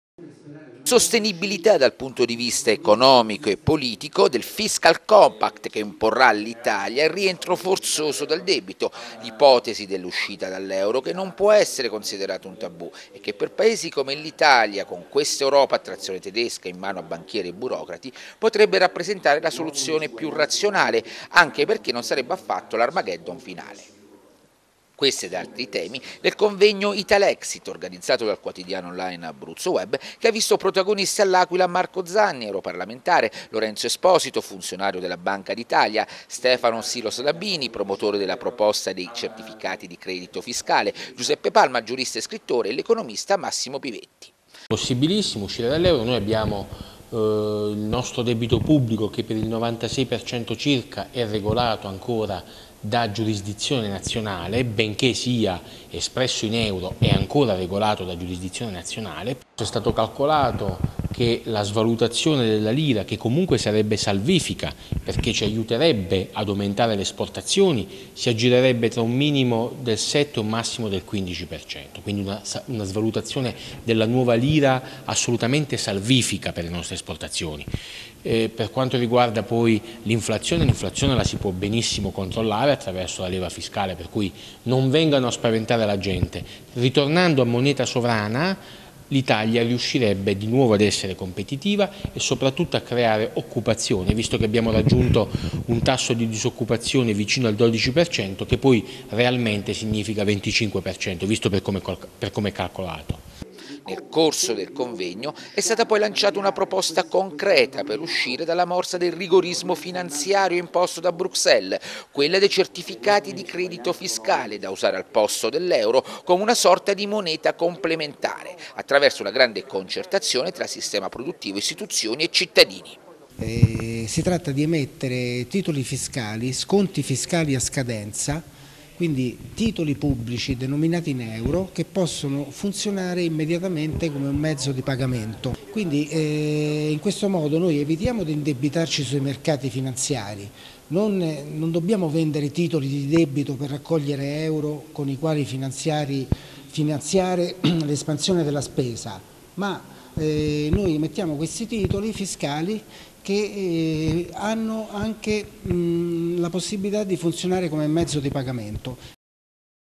ItalExit a L'Aquila
A L'Aquila si è svolto un convegno in cui si è affrontato il tema dell'uscita dall'euro dell'Italia. Un uscita, dicono, che è possibile con il ritorno alla lira